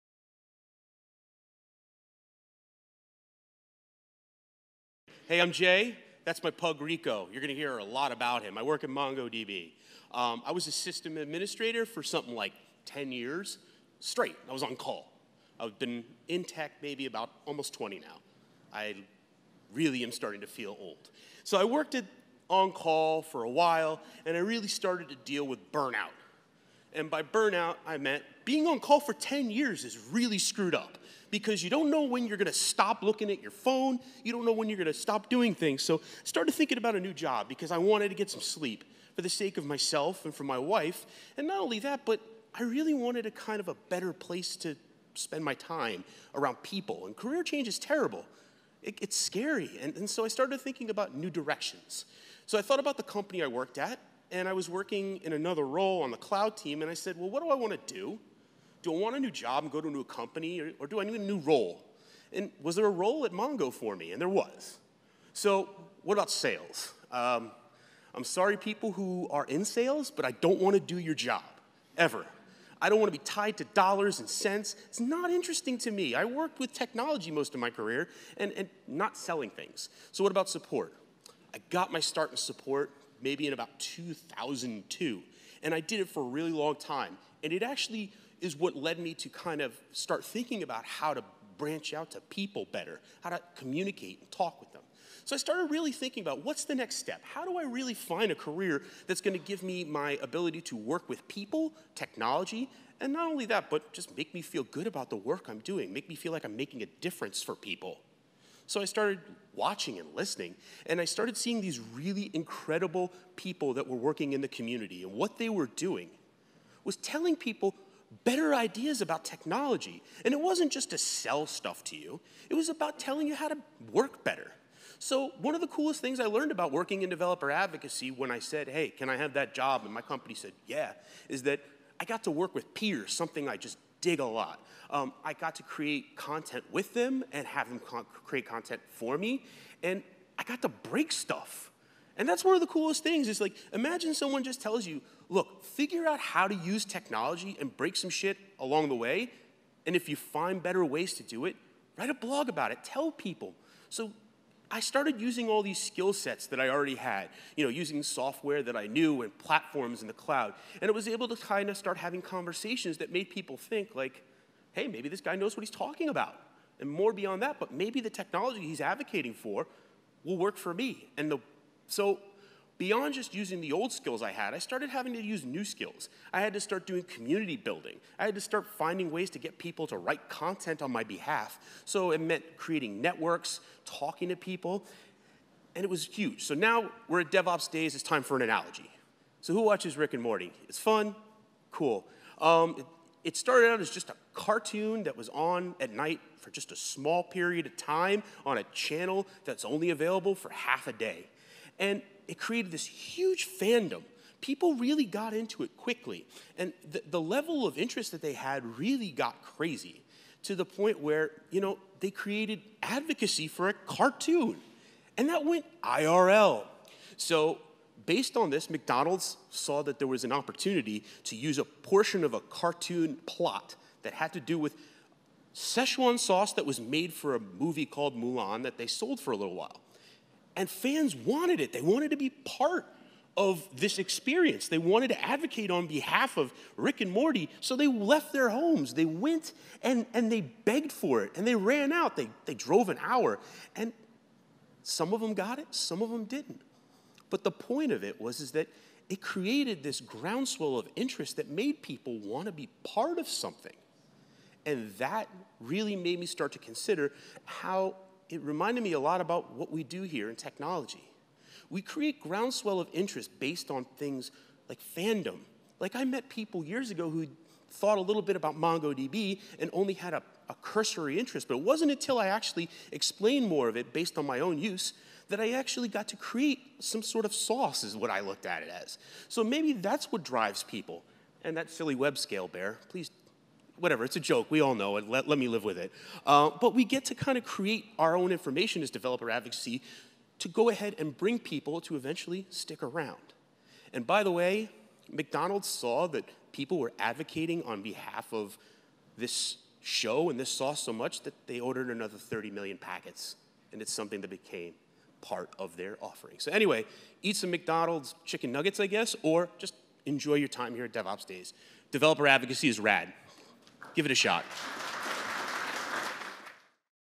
On Being A Developer Advocate - Jay Gordon
DevOps professionals are required for teams to advocate on behalf of software. Without Developer Advocates, code exists as text in a repository. By spreading the value of good information we can help good practices become part of the DevOps experience.